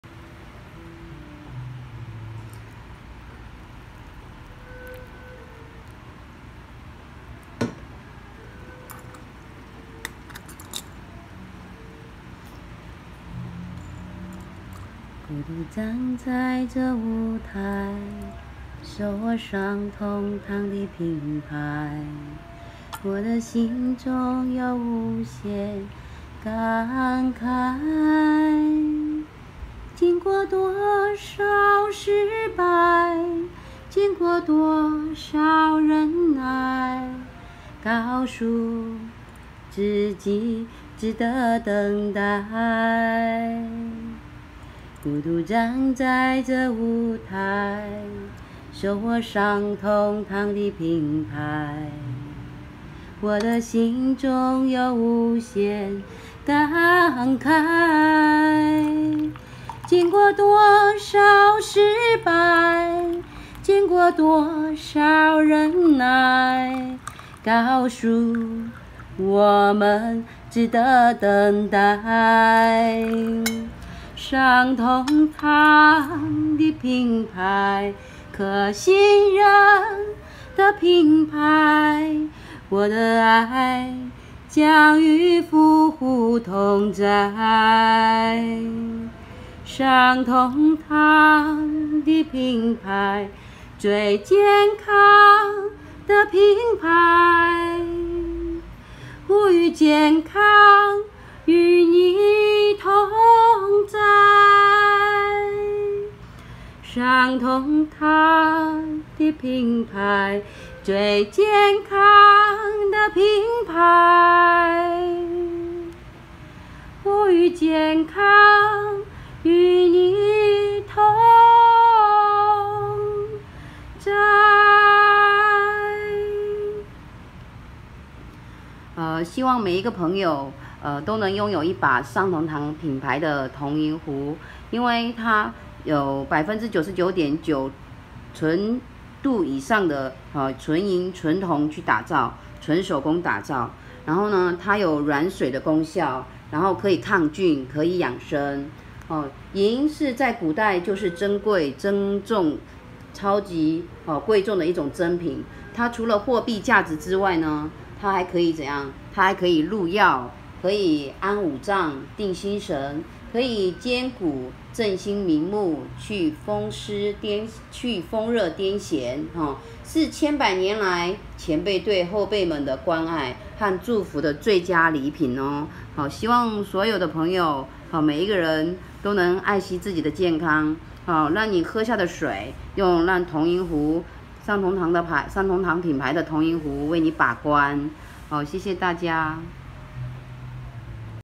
《尚銅堂》彩樺秀一秀
秀一秀😊，今天來點不一樣的😊。
一次心，眼，手，口，的併用😊。
送女兒上學途中，想到的曲😊，今天把它用上試試😊🌹❤️。